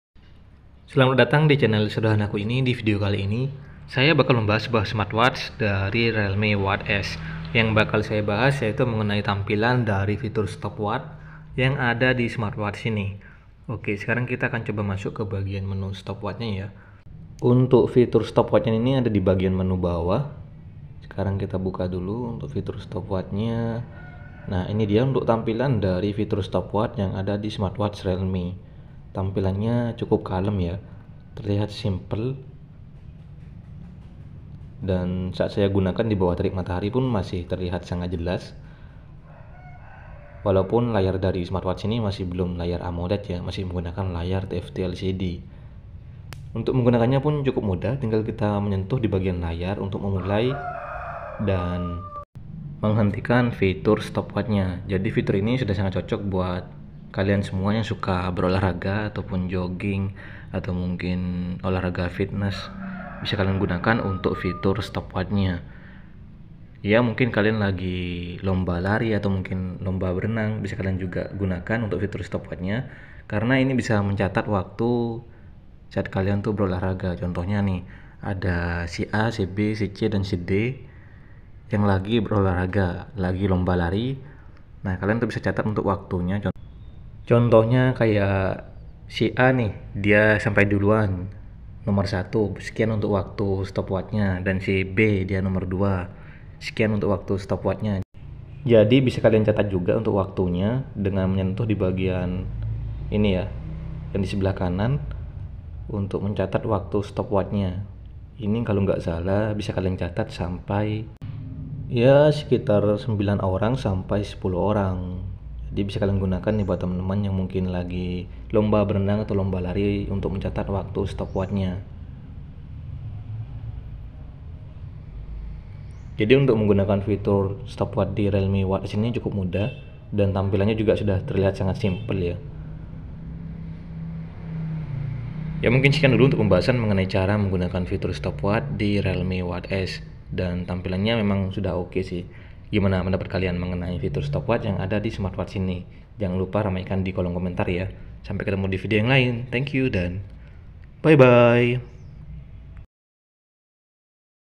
🌀 Tampilan Dan Cara Pakai Fitur Stopwatch Realme Watch S || Cocok Buat Yang Suka Olahraga
Nah buat yang tertarik untuk membeli smartwatch realme, mungkin realme watch s bisa menjadi salah satu pilihan buat kalian yang pengin beli realme watch.
untuk fitur udah oke dikelasnya, ada fitur stopwatch juga yang bisa kalian gunakan tipis tipis saat beraktivitas seperti olahraga dan kegiatan lainnya